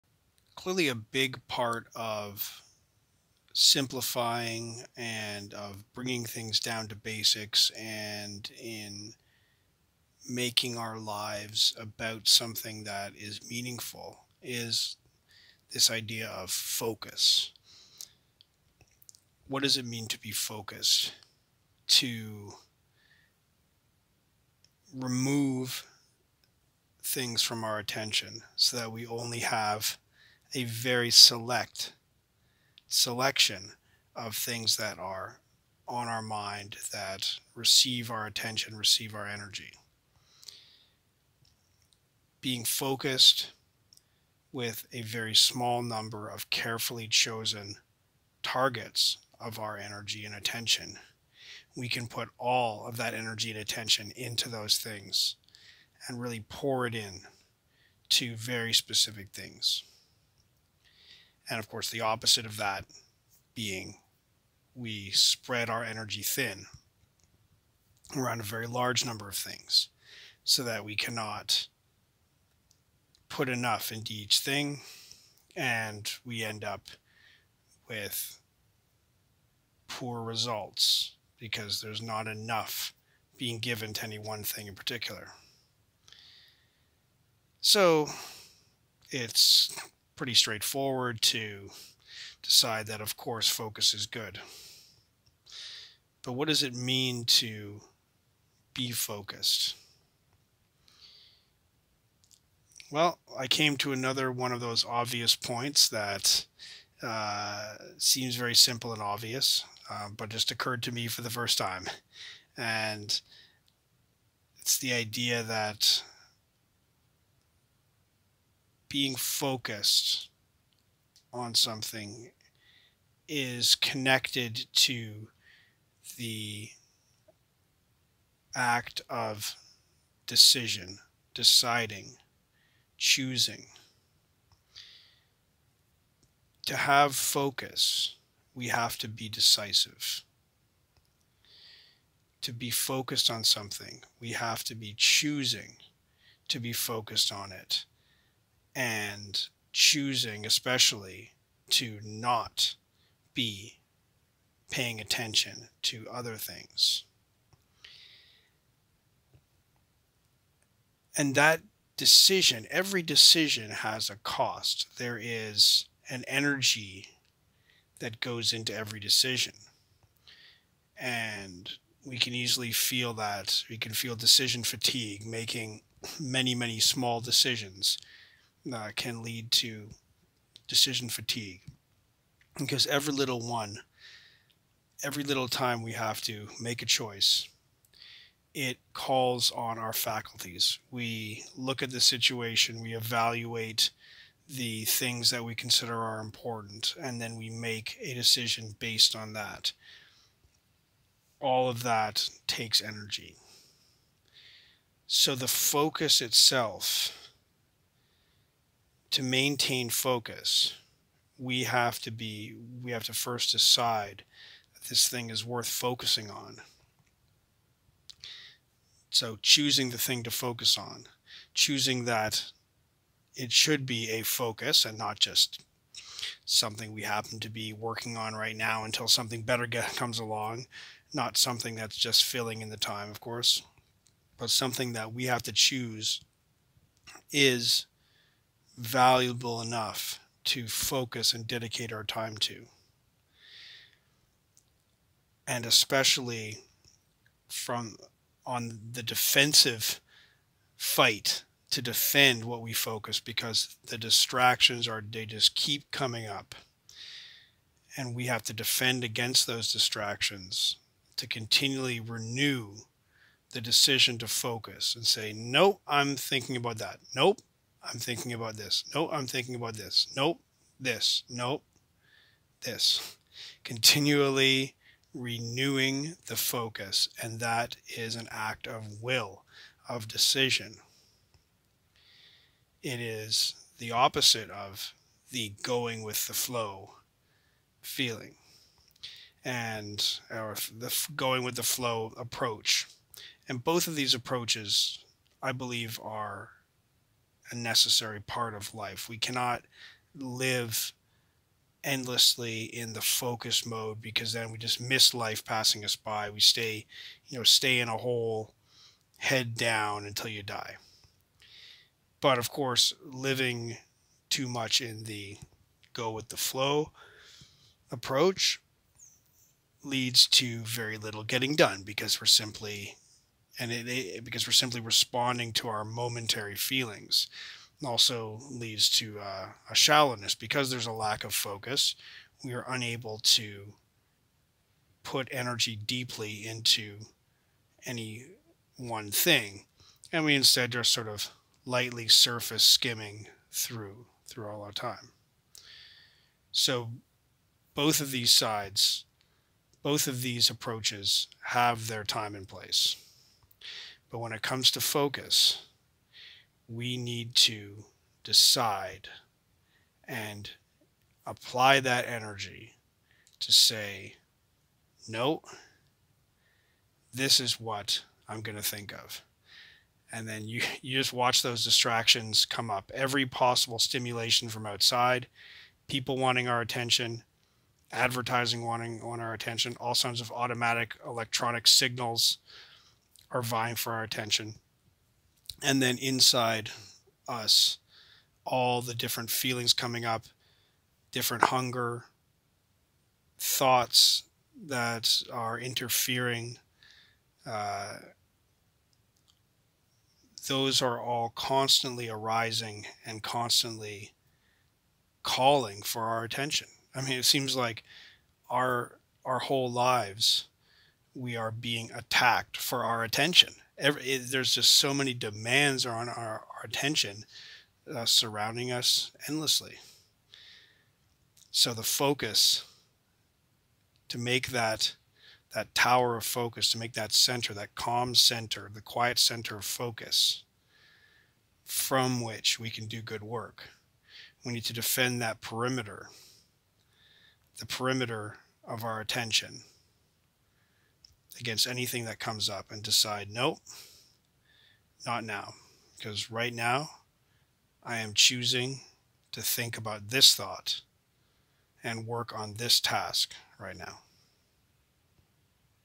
Spreading too thin: Defend your attention, defend your energy
Clearly a big part of simplifying and of bringing things down to basics, and in making our lives about something that is meaningful, is this idea of focus.
What does it mean to be focused? To remove things from our attention, so that we only have a very select selection of things that are on our mind, that receive our attention, receive our energy.
Being focused with a very small number of carefully chosen targets of our energy and attention, we can put all of that energy and attention into those things, and really pour it in to very specific things.
And of course, the opposite of that being we spread our energy thin around a very large number of things, so that we cannot put enough into each thing, and we end up with poor results, because there's not enough being given to any one thing in particular.
So it's pretty straightforward to decide that, of course, focus is good.
But what does it mean to be focused? Well, I came to another one of those obvious points that seems very simple and obvious but just occurred to me for the first time, and it's the idea that being focused on something is connected to the act of decision.
Deciding.
Choosing.
To have focus, we have to be decisive.
To be focused on something, we have to be choosing to be focused on it, and choosing, especially, to NOT be paying attention to other things.
And that decision- every decision has a cost.
There is an energy that goes into every decision.
And we can easily feel that.
You can feel decision fatigue.
Making many, many small decisions can lead to decision fatigue, because every little one, every little time we have to make a choice, it calls on our faculties.
We look at the situation, we evaluate the things that we consider are important, and then we make a decision based on that.
All of that takes energy.
So the focus itself- to maintain focus, we have to first decide that this thing is worth focusing on.
So choosing the thing to focus on, choosing that it should be a focus, and not just something we happen to be working on right now until something better comes along, not something that's just filling in the time, of course, but something that we have to choose is valuable enough to focus and dedicate our time to.
And especially on the defensive fight.
to defend what we focus, because the distractions just keep coming up, and we have to defend against those distractions, to continually renew the decision to focus, and say, no, I'm thinking about that.
No, I'm thinking about this.
No, this.
No, this.
Continually renewing the focus.
And that is an act of will, of decision.
It is the opposite of the going-with-the-flow feeling, or the going-with-the-flow approach.
And both of these approaches, I believe, are a necessary part of life.
We cannot live endlessly in the focus mode, because then we just miss life passing us by.
We stay, you know, stay in a hole, head down, until you die.
But, of course, living too much in the go-with-the-flow approach leads to very little getting done, because we're simply responding to our momentary feelings.
It also leads to a shallowness.
Because there's a lack of focus, we are unable to put energy deeply into any one thing, and we instead just sort of lightly surface skimming through all our time.
So both of these sides, both of these approaches, have their time and place.
But when it comes to focus, we need to decide, and apply that energy to say, no, this is what I'm gonna think of.
And then you just watch those distractions come up.
Every possible stimulation from outside, people wanting our attention, advertising wanting our attention, all sorts of automatic electronic signals are vying for our attention.
And then inside us, all the different feelings coming up, different hunger, thoughts that are interfering, those are all constantly arising and constantly calling for our attention.
I mean, it seems like our whole lives, we are being attacked for our attention.
There's just so many demands on our attention surrounding us endlessly.
So the focus, to make that tower of focus, to make that centre, that calm centre, the quiet centre of focus from which we can do good work, we need to defend that perimeter, the perimeter of our attention, against anything that comes up, and decide, no, not now, because right now, I am choosing to think about THIS thought and work on THIS task right now.